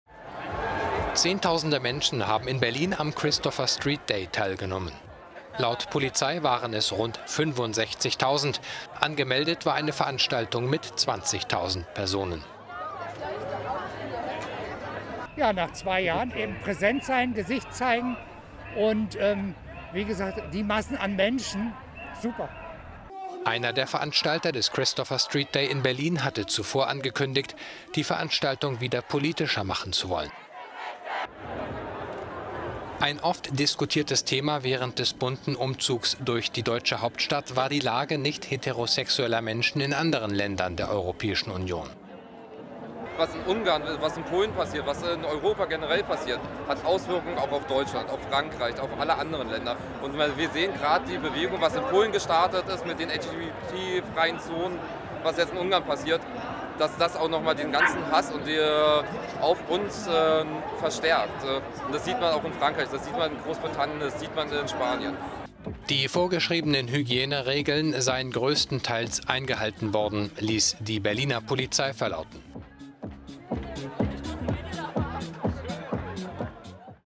Bunt und mit klarer Botschaft: Christopher Street Day in Berlin
Laut Polizei nahmen 65 000 Menschen an dem Umzug durch die deutsche Hauptstadt teil. Ein Thema: Die Lage nicht-heterosexueller Menschen in anderen Ländern der Europäischen Union.